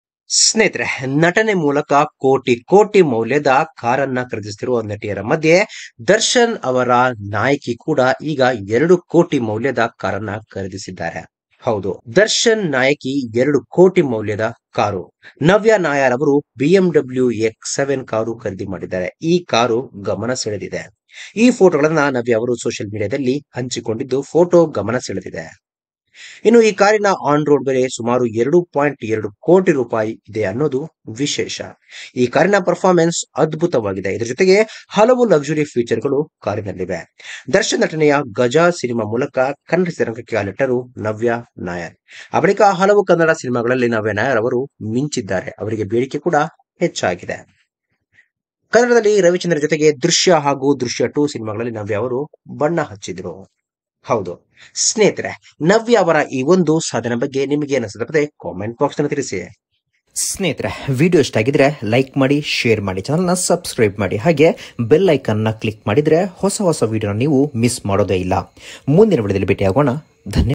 2 ಕೋಟಿ ಬೆಲೆಯ ಕಾರು ಖರೀದಿಸಿದ ದರ್ಶನ್ ನಟಿ! ಯಾರು ಆ ನಟಿ ಗೊತ್ತಾ?!!
Darshan Actress bought 2 crore car!